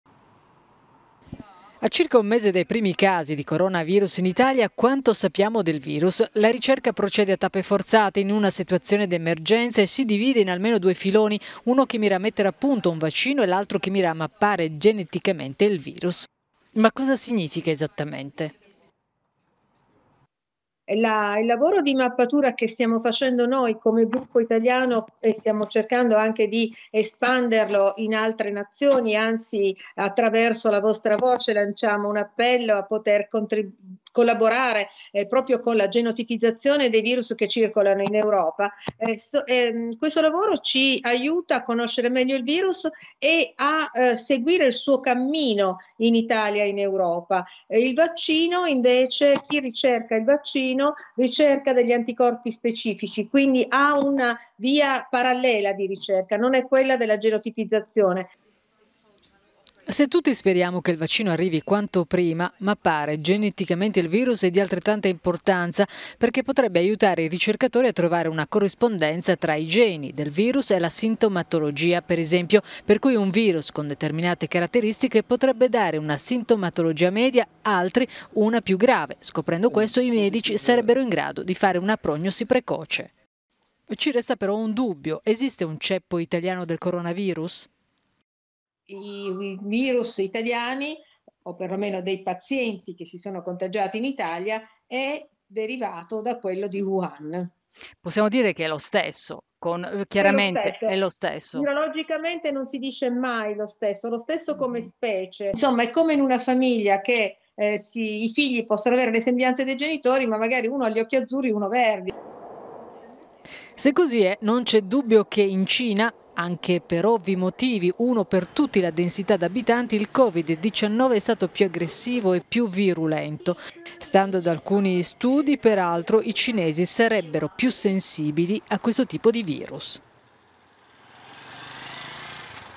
Coronavirus: perché la ricerca è fondamentale in queste ore
Le prossime ore sono fondamentali per dire se le misure di contenimento del coronavirus hanno funzionato. A noi di euronews restano ancora tanti dubbi sul decorso dell'infenzione, qualcuno l'abbiamo chiarito parlando con MariaRitaGismondo…